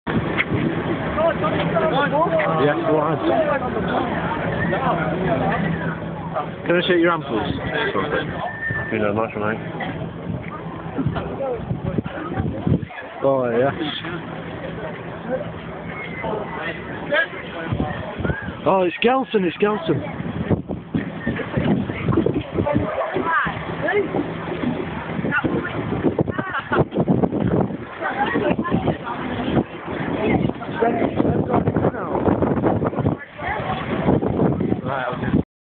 Nedum Onuoha
Nedum Onuoha talks to fans after 0-0 draw with bolton.